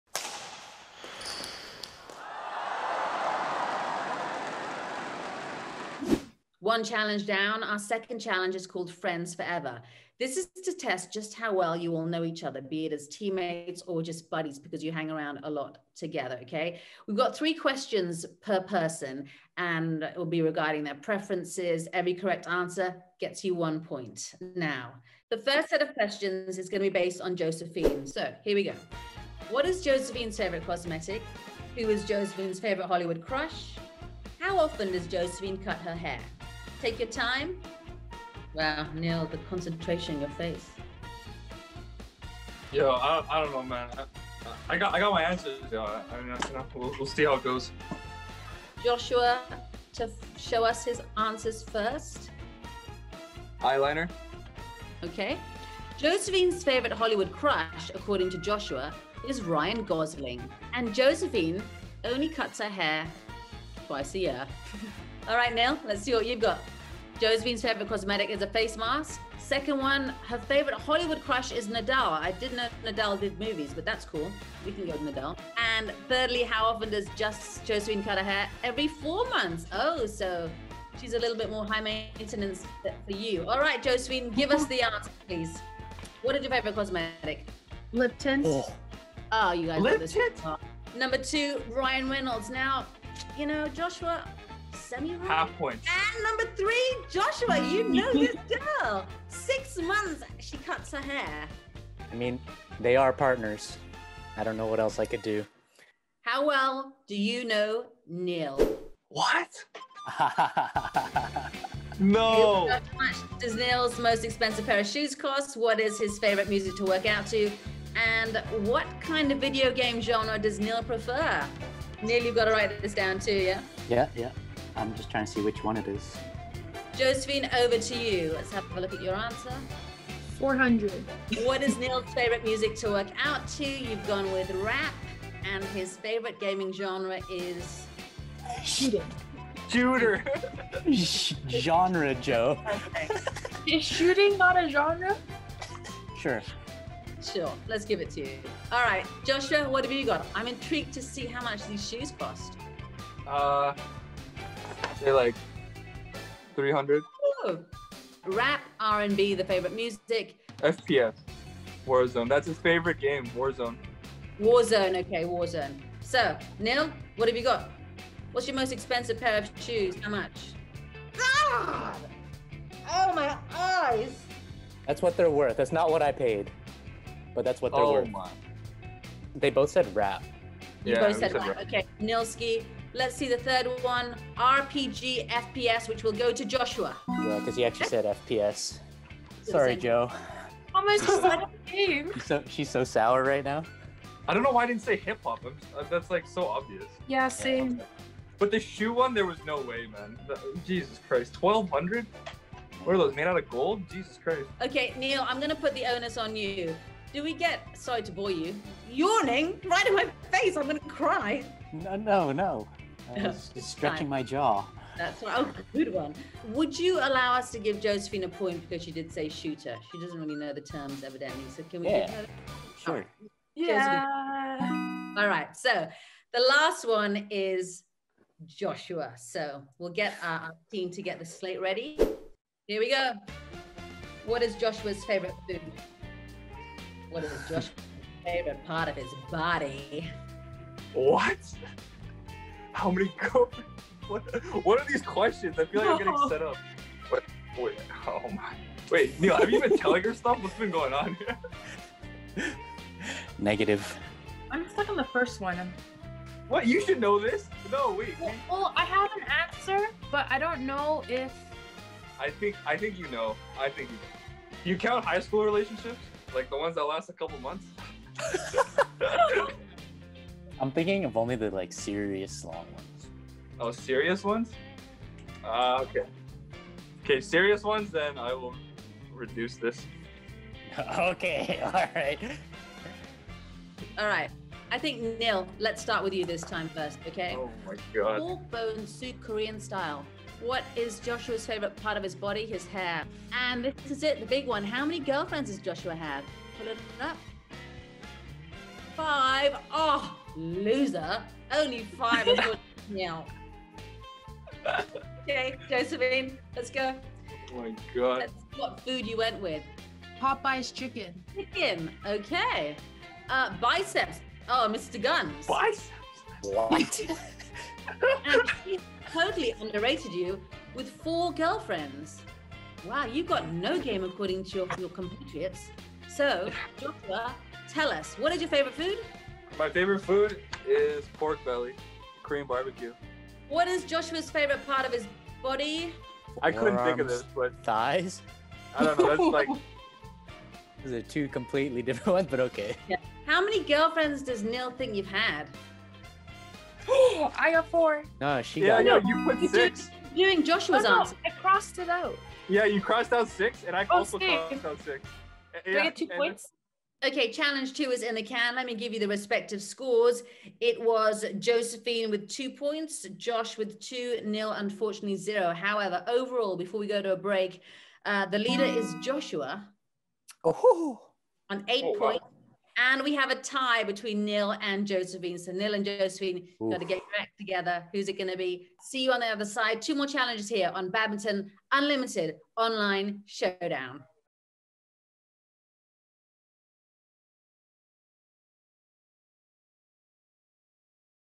Badminton Unlimited | Friends Forever | BWF 2021
Canada's Joshua Hurlburt-Yu, Josephine Wu and Nyl Yakura discover some personal and quirky preferences of their teammates through this fun game.